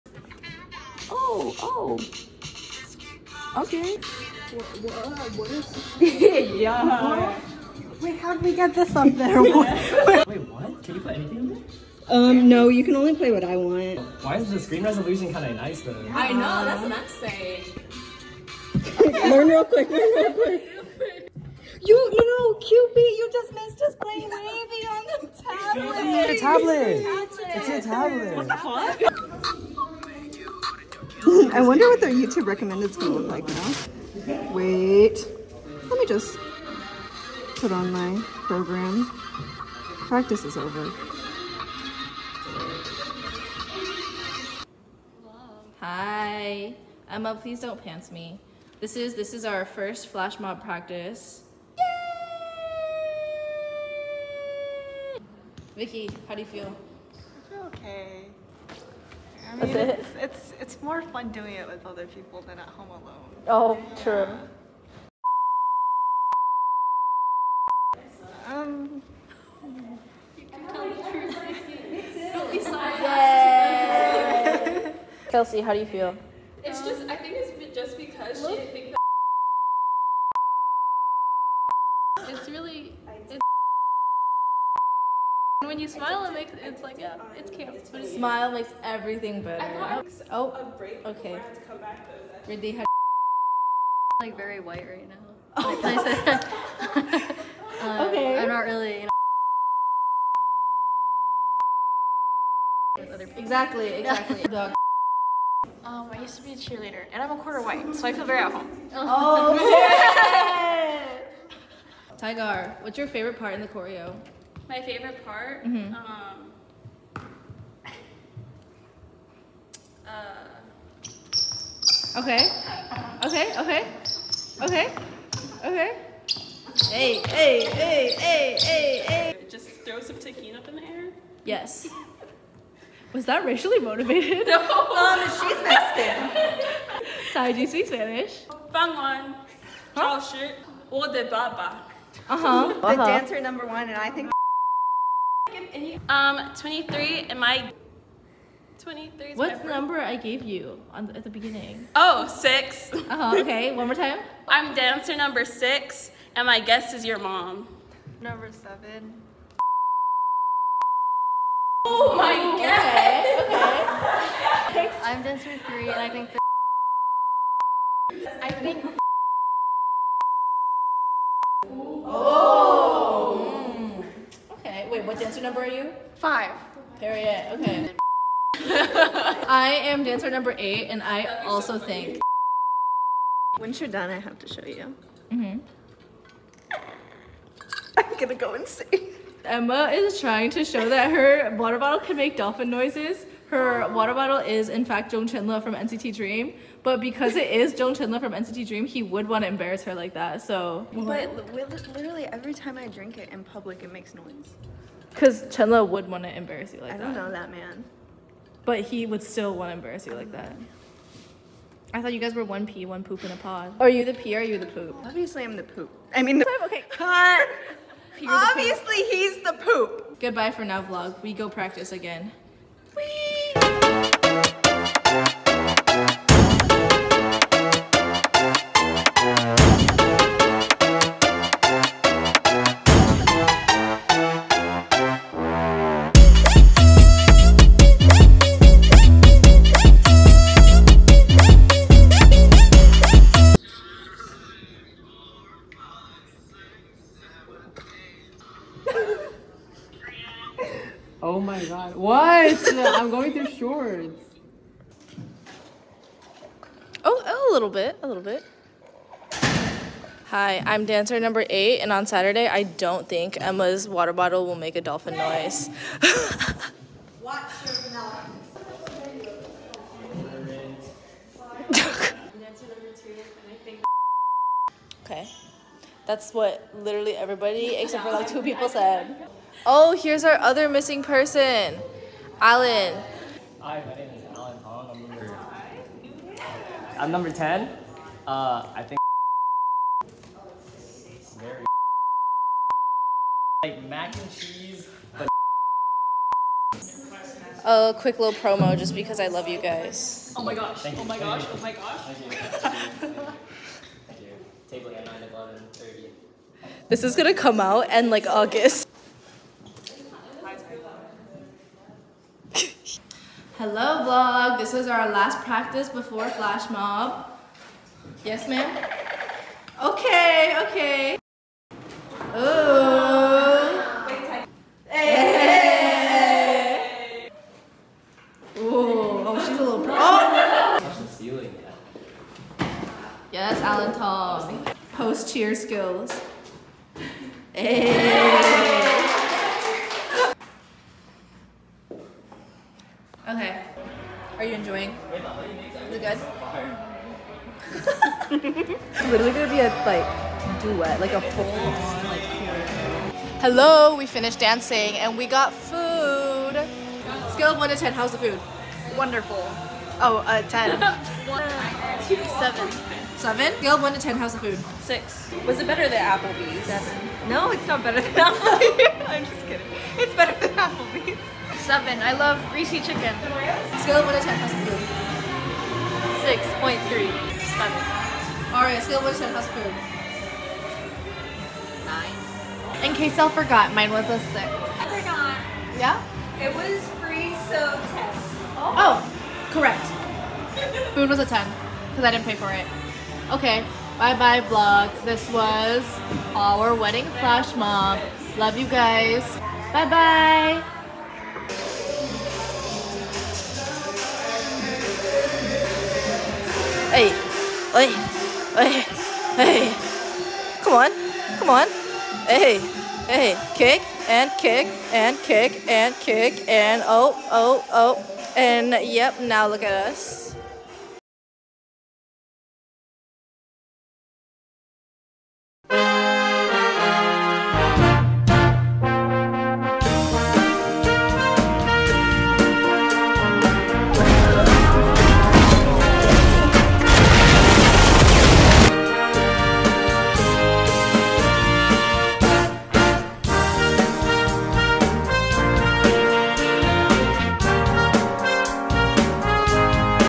RDWedding Crashers - RDSummer Vlog Ep. 2
Watch us crash a wedding 🫣 Jk 🤭 Watch us be HIRED wedding flashmobers!!

My sincerest apologies to our 4 fans about the late upload 😞 It won't happen again - Emma 🐈‍⬛😈